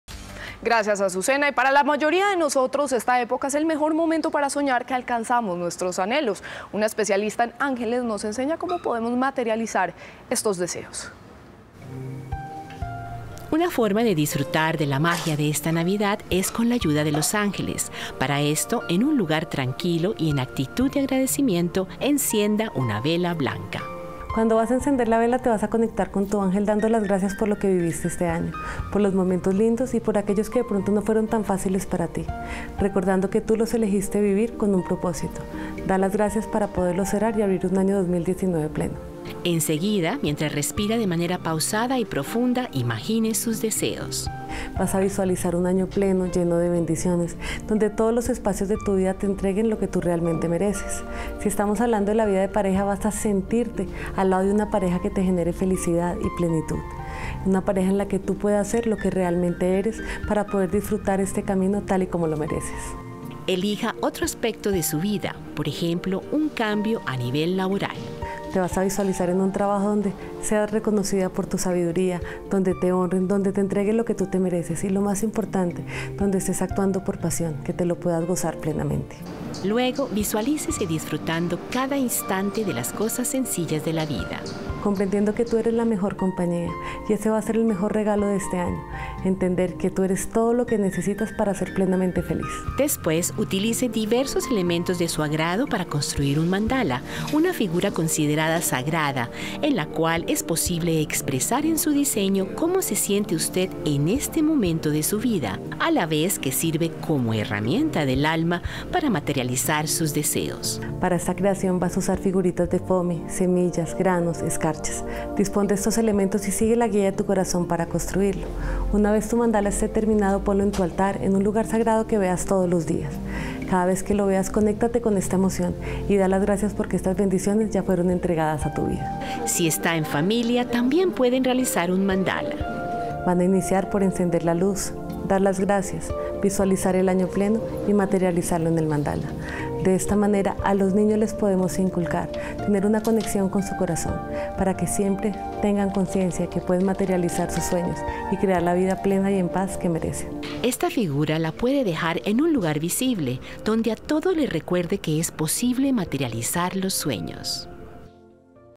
Los ángeles y la Navidad: especialista le enseña cómo conectarse con ellos
Para empezar, en un lugar tranquilo y en actitud de agradecimiento, encienda una vela blanca.